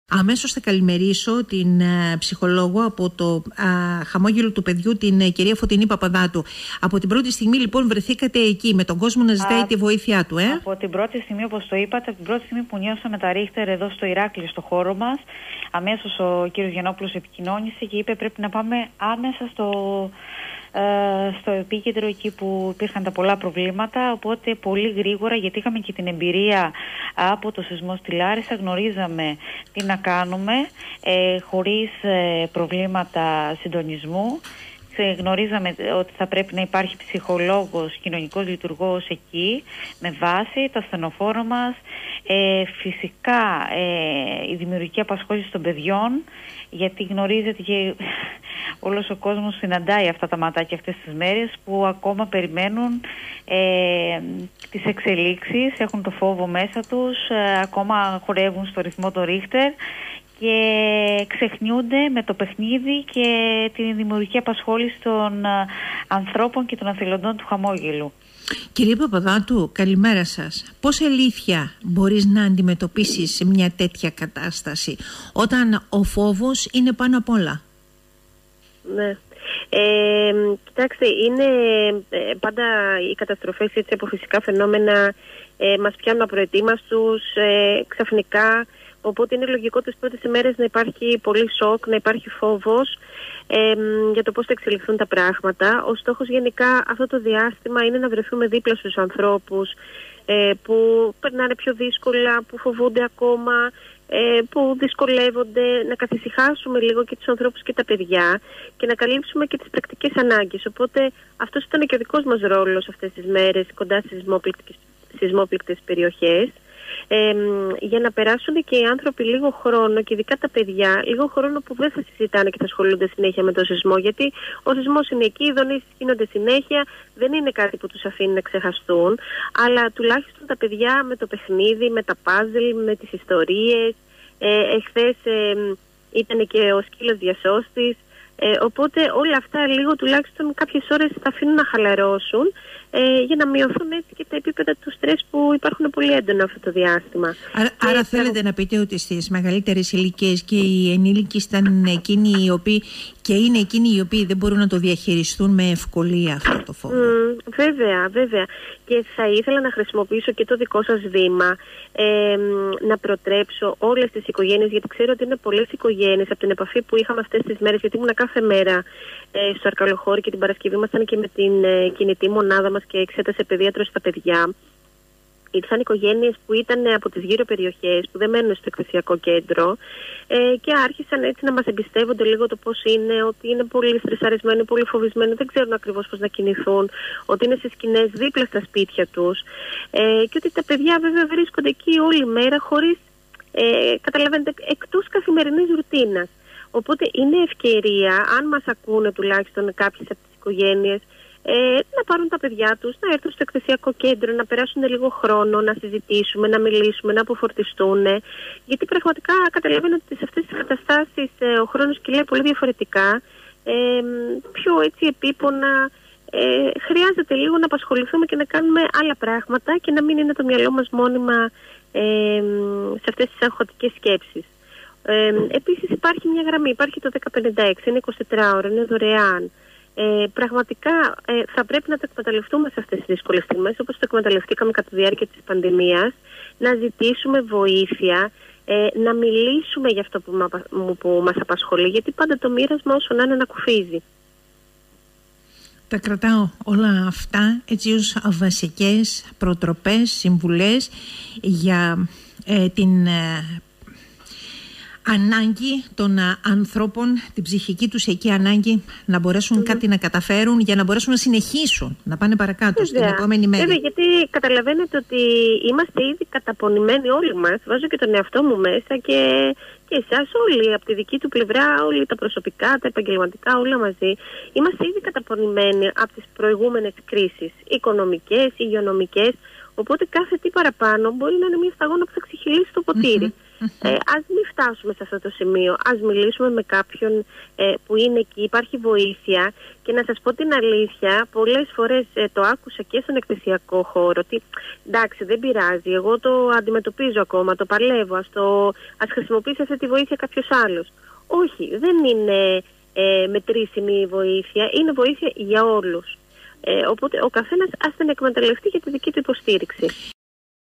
Φ. Παπαδάτου: Σοκ και φόβο νοιώθουν οι σεισμόπληκτοι
Τους προτείνω να επικοινωνήσουν με το 1056 στο «Χαμόγελο του Παιδιού» για την ψυχολογική στήριξη τους
Στο πλευρό των σεισμόπληκτων από την πρώτη στιγμή βρέθηκε η μονάδα του οργανισμού στην προσπάθεια στήριξης των ανθρώπων που έχουν πληγεί. Κι αν τα παιδιά μπορούν να απασχοληθούν και να ξεχαστούν για λίγο για τους ενήλικες τα βιώματα είναι διαφορετικά και δύσκολα όταν η γη σείεται συνεχώς κάτω από τα πόδια σου, λέει στον 98.4 η ψυχολόγος από το  «Χαμόγελο του Παιδιού» Φωτεινή Παπαδάτου.